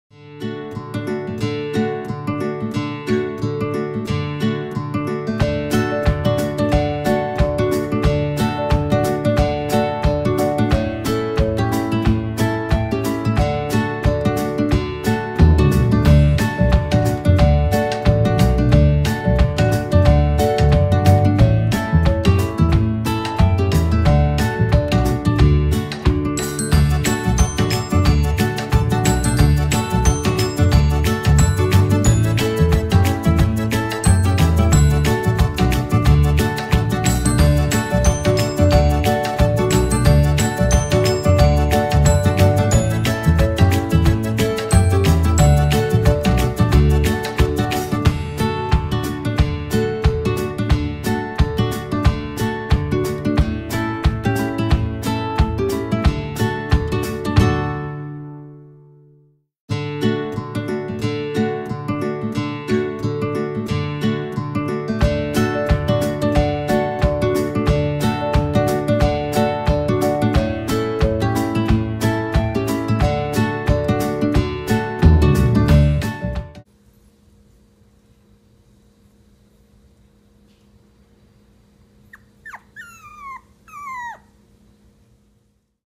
Olivia Pizarra un sábado en la mañana
Corriendo y brincando en el césped. Felicidad, descripción gráfica.